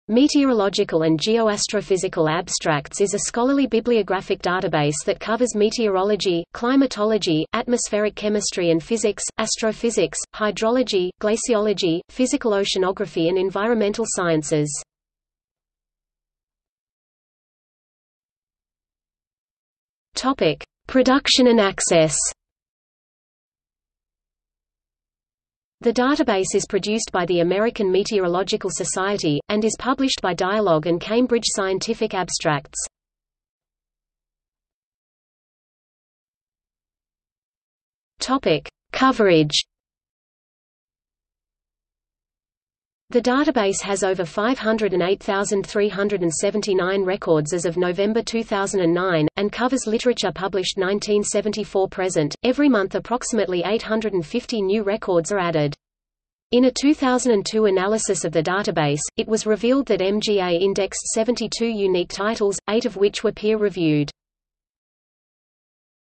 Meteorological & Geoastrophysical Abstracts | Wikipedia audio article
00:00:16 Production and Access
00:00:34 Coverage

- Socrates

SUMMARY
Meteorological & Geoastrophysical Abstracts is a scholarly bibliographic database that covers meteorology, climatology, atmospheric chemistry and physics, astrophysics, hydrology, glaciology, physical oceanography and environmental sciences.